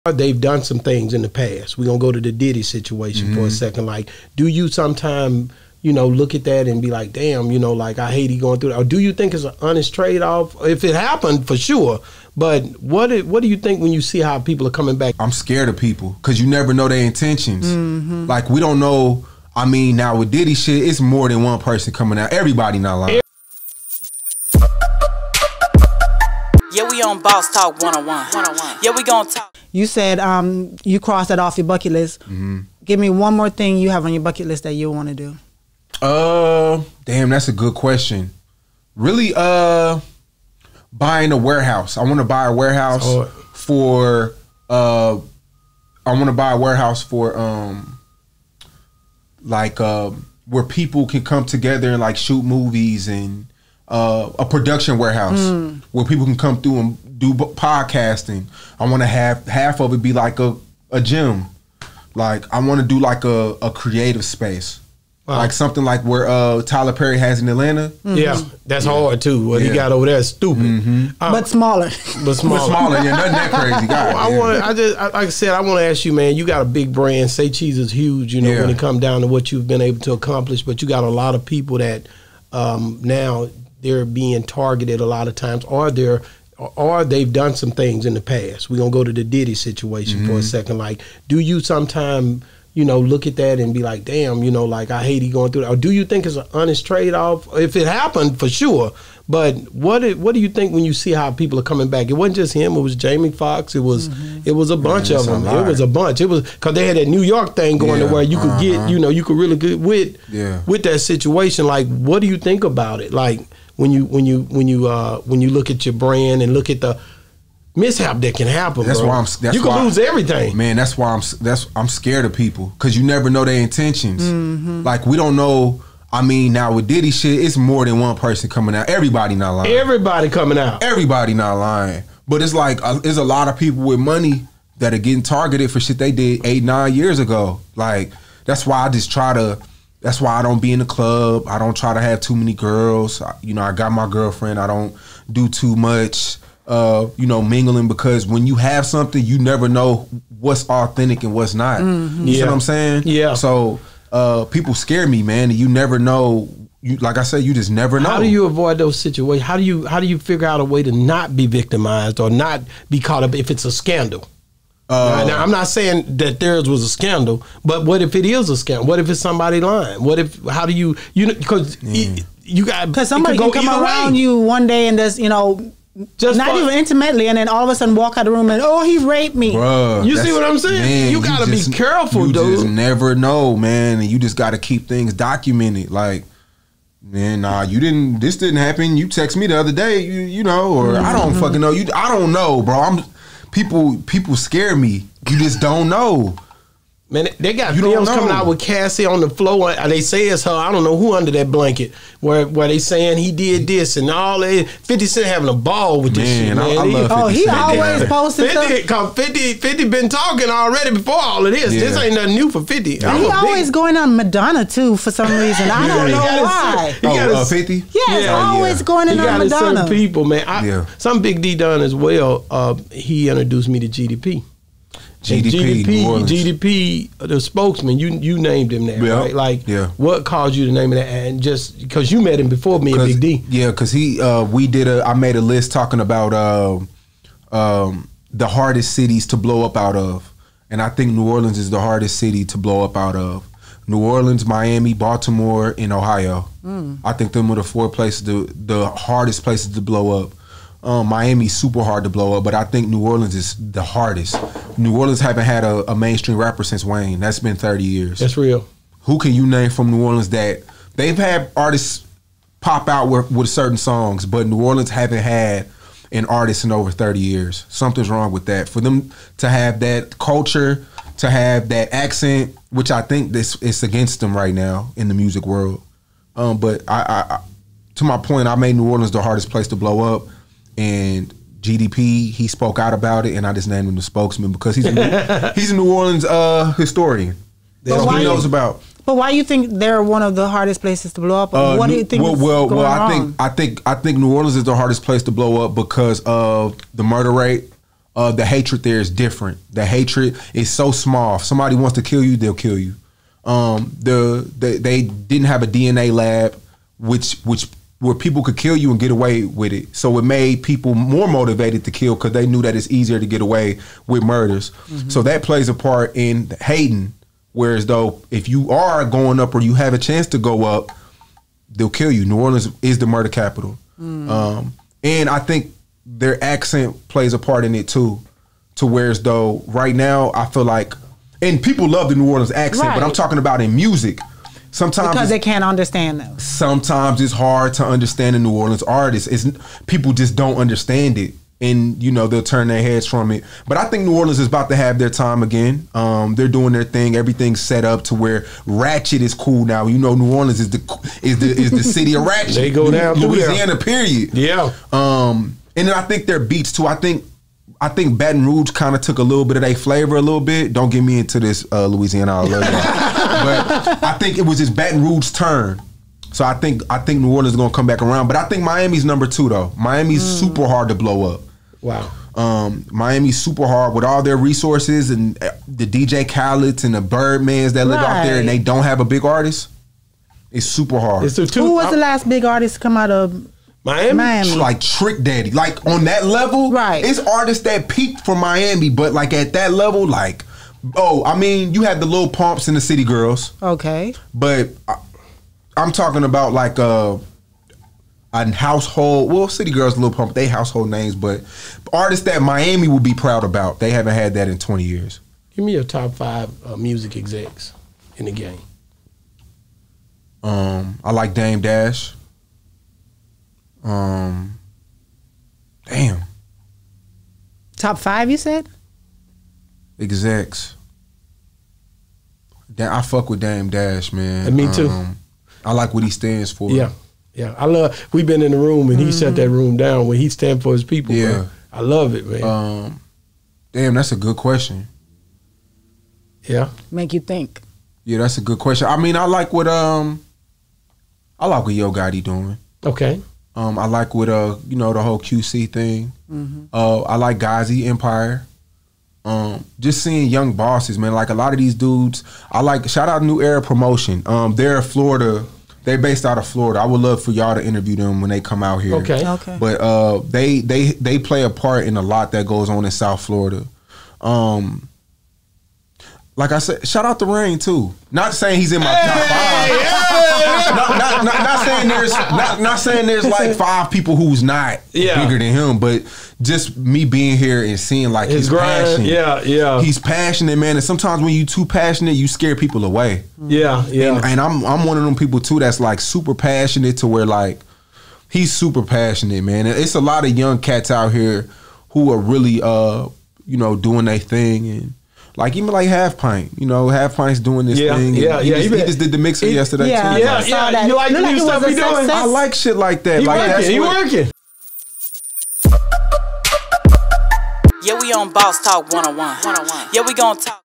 Shawn Cotton on P Diddy Assault Allegations, 50 Cent Buying Revolt Top 5 Music EXEC Geedy P (Part 4)
Executive Producer: Chasity Swilley
Producer: Stephanie Smith
Video Manager: Dennis Cooper
Content Manager: Shamariah Smith
Public Relations: Tre' Smith